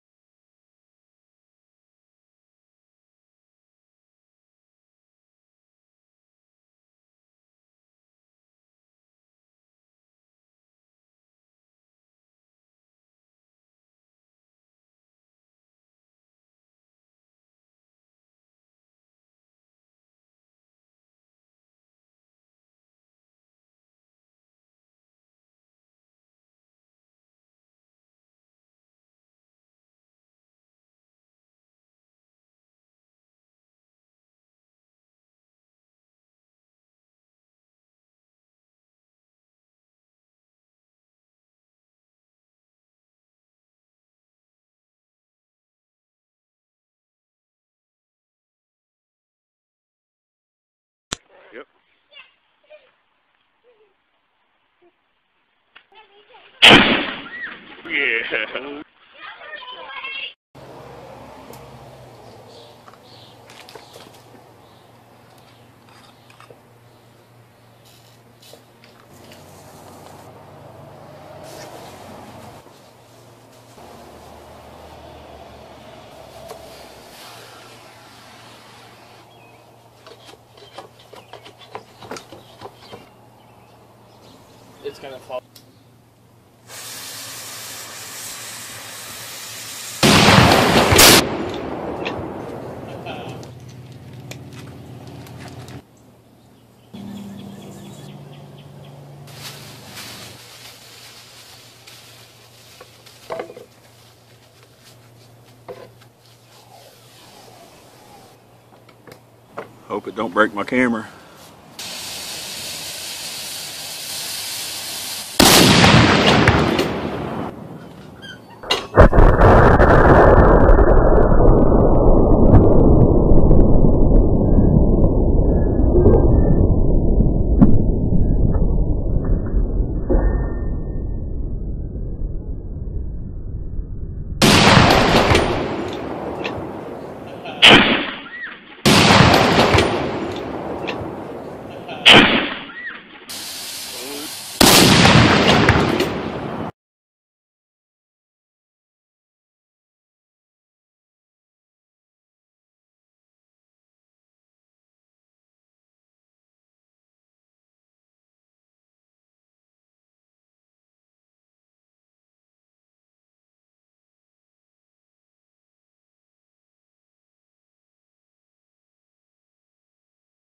Shooting My Homemade Cannon. build pics and firing compilation homemade cannon homemade gun
I dug up some old pics of the build and the very first test fire video. Fun cannon. I built this cannon in 2006 Check out my cannon/gun playlist.